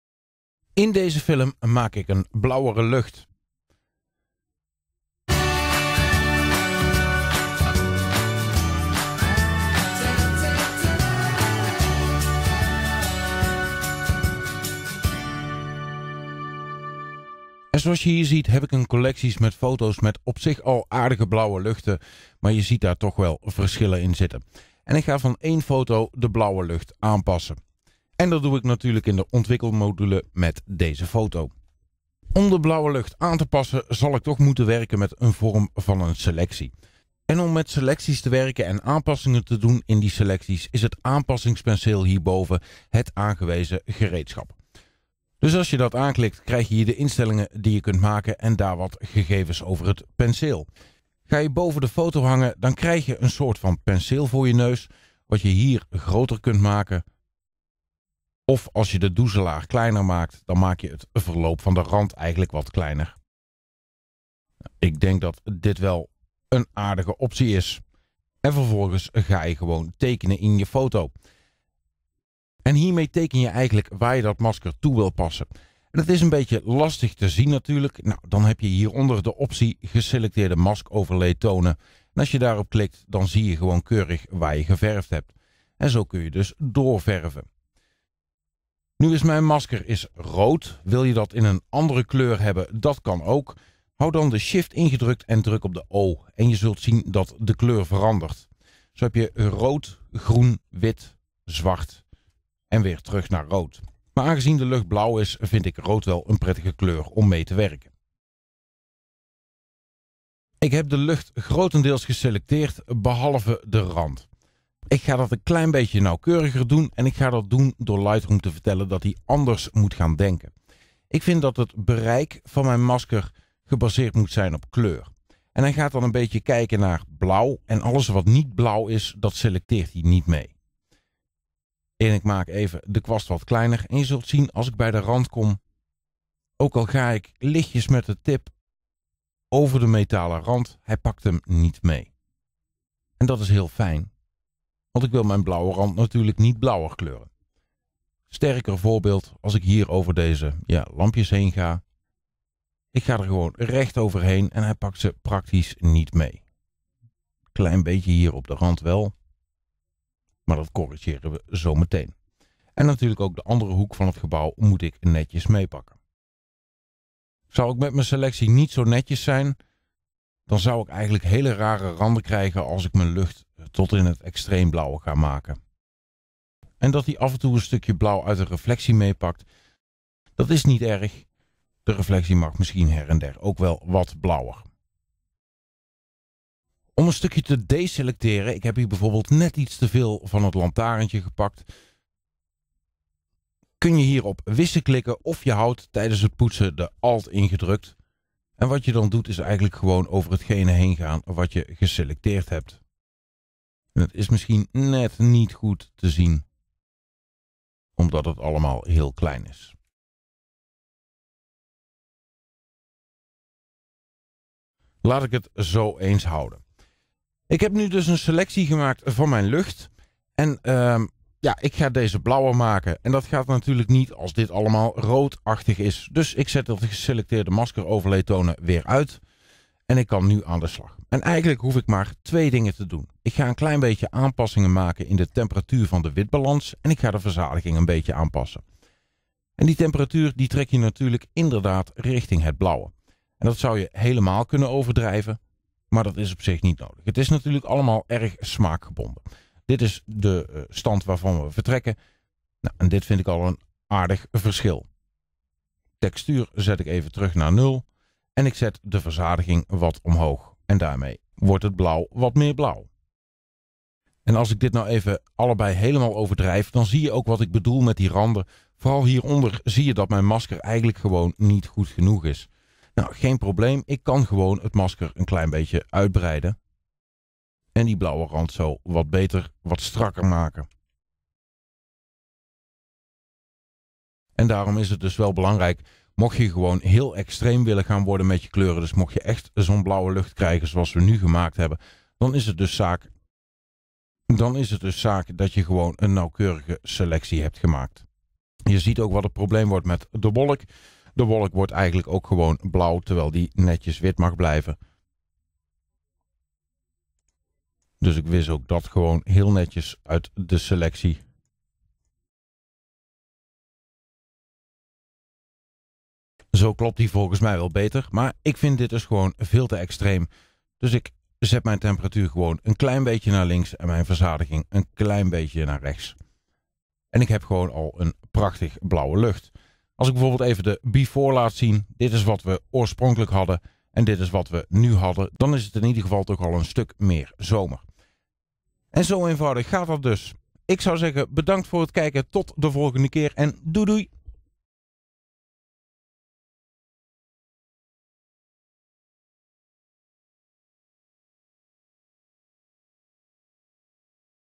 Blauwere luchten (Lightroom Classic)
Je hebt blauwe luchten en je hebt blauwe luchten. Maar met een selectie maak jij in Lightroom de lichten precies zo blauw als dat jij ze wilt hebben.

In deze film laat ik je zien hoe het allemaal werkt. Ik laat zien hoe je een goede selectie maakt en wat uiteindelijk de trucjes zijn om je lucht net even dat dieper blauw te maken van een echter zomerse dag.